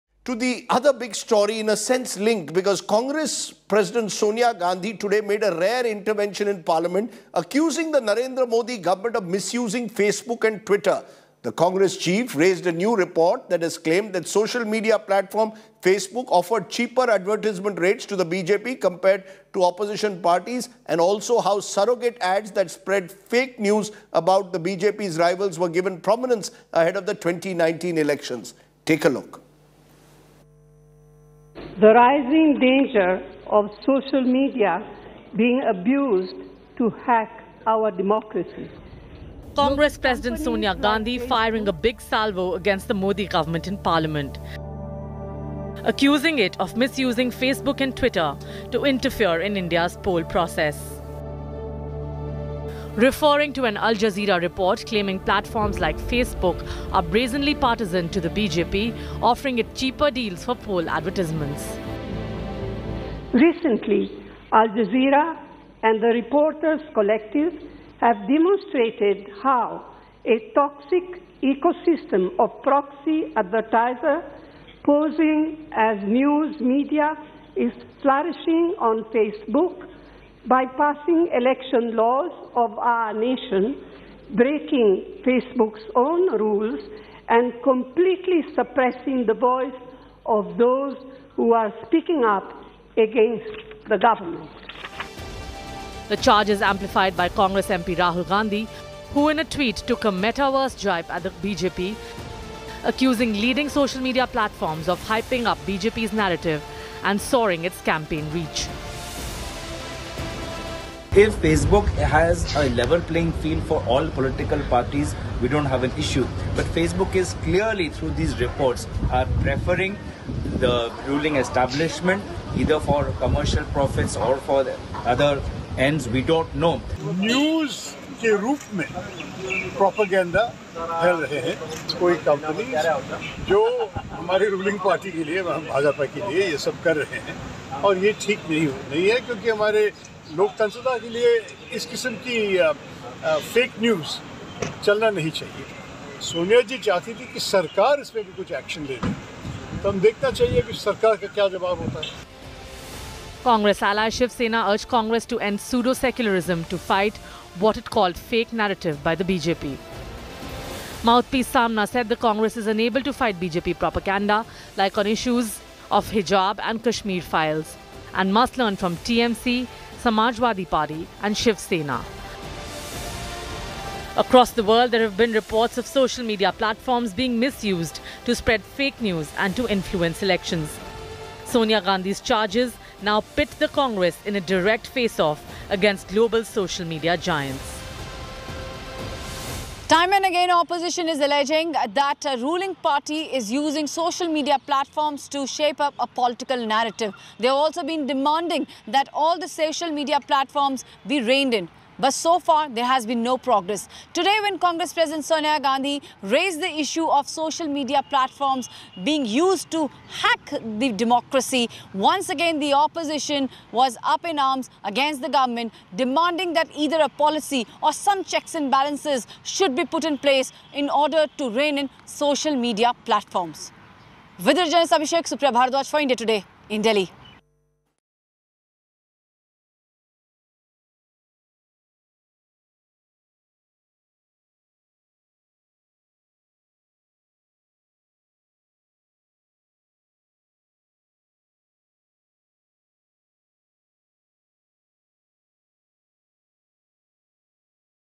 Social Media Being Abused To Hack Our Democracy, Says Congress President Sonia Gandhi In Lok Sabha
Speaking in the Lok Sabha on Wednesday, Congress interim president Sonia Gandhi said that social media is being abused to "hack our democracy".

She said that social media platforms like Facebook and Twitter are being manipulated to set political narratives in India.

"Global companies like Facebook and Twitter are increasingly being used to shape political narratives by leaders, parties and their proxies. This show the connivance of the ruling establishment with these social media platforms to set political narratives, which is not helpful for democracy and the democratic structure," Sonia Gandhi said.

Here are the political reactions on Sonia Gandhi's remark.